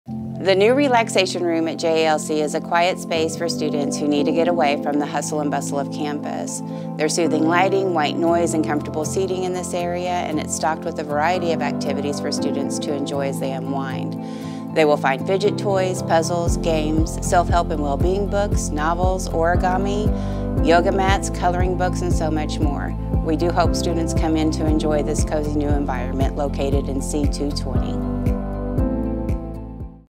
Relaxation Room
Need to get away from all the hustle and bustle of college life? Unwind in our Relaxation Room in C220! With soothing lighting, activities, comfortable seating & more, we’re here to help ease your mind. Visit us today!